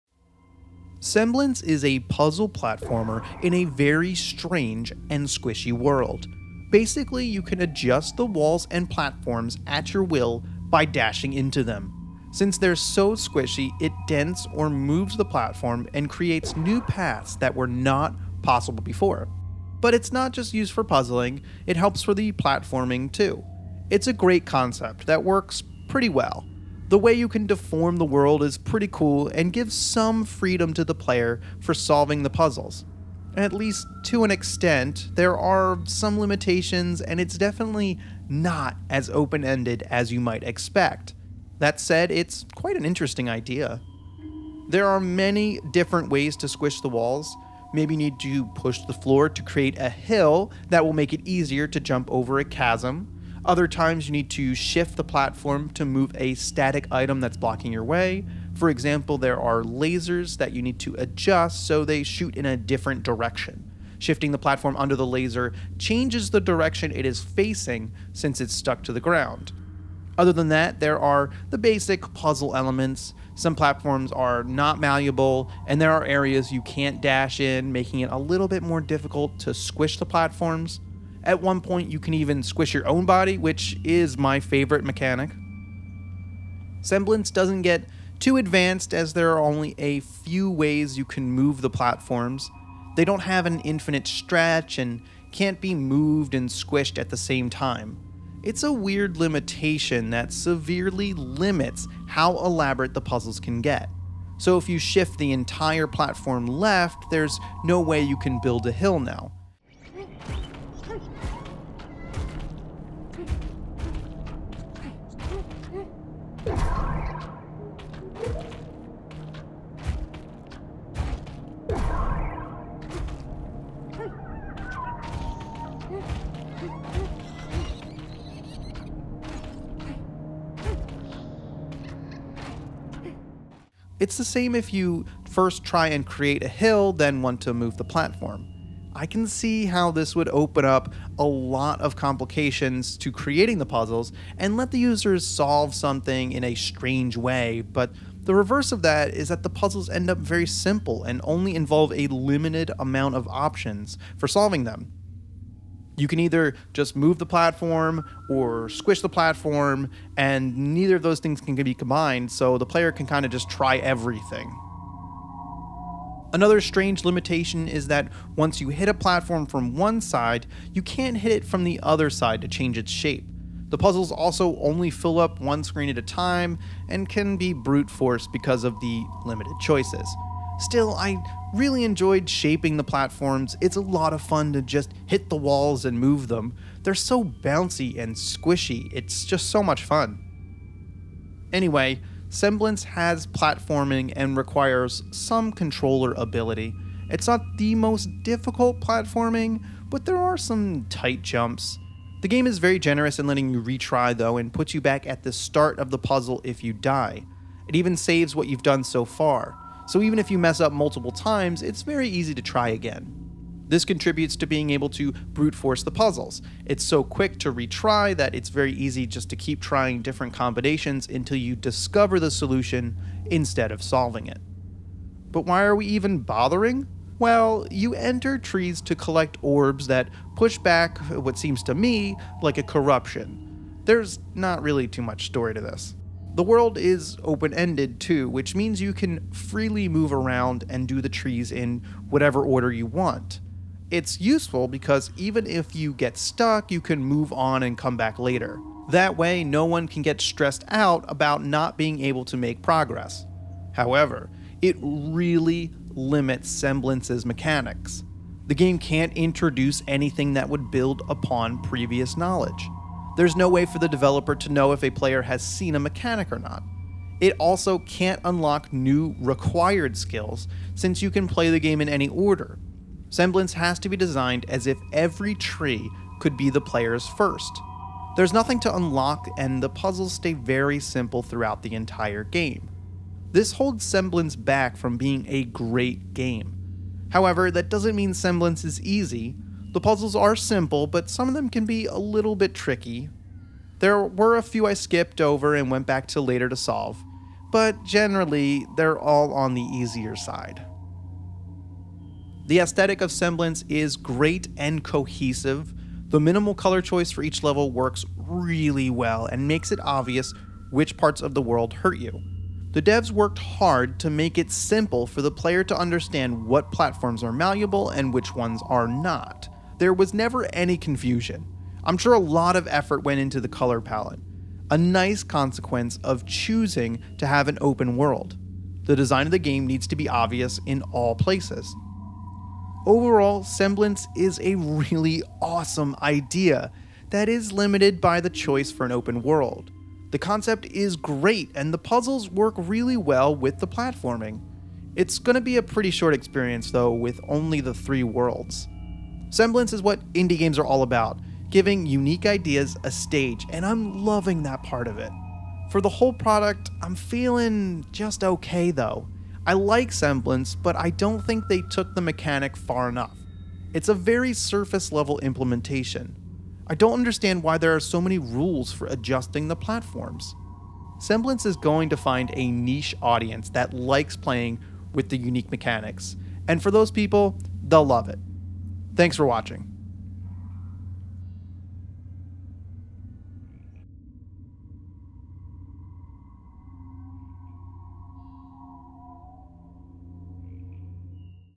Semblance Review
Semblance review, a squishy puzzle platformer by Nyamakop. Deform the world to create a path to help push back the corruption!

Review copy provided free of charge.

This curator thing is awesome!